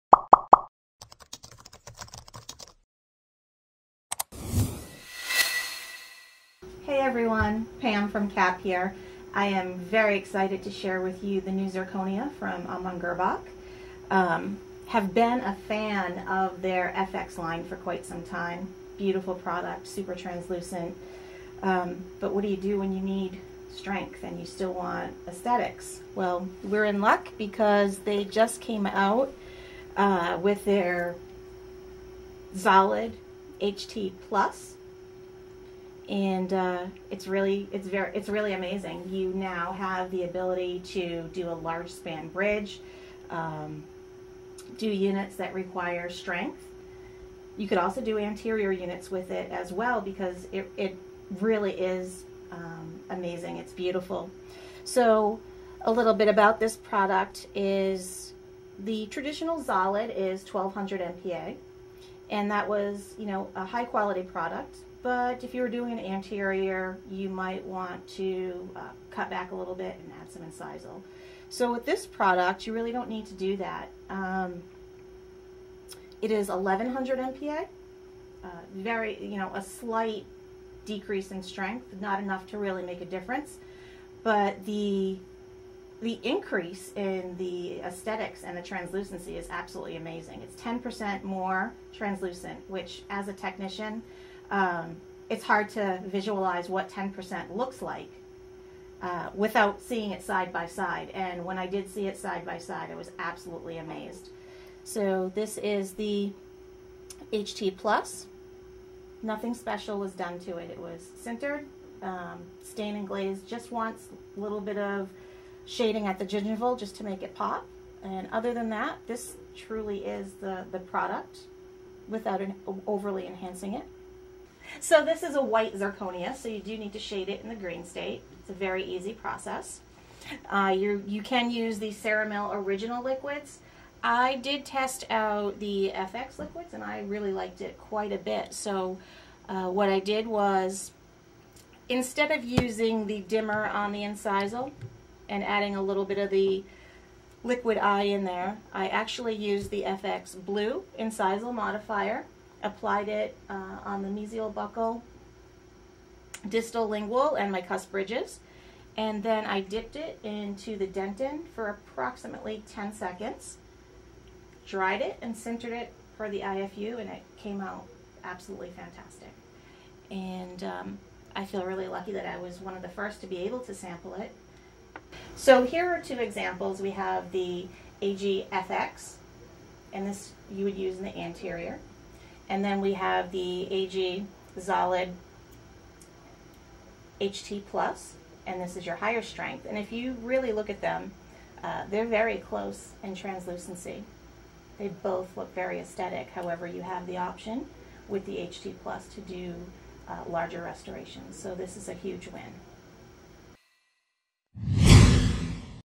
Dental Lab Life: Amann Girrbach Zolid HT+
In this episode of Dental Lab Life! Pam Hanneman goes over the new zirconia from Amann Girrbach, the Zolid HT+.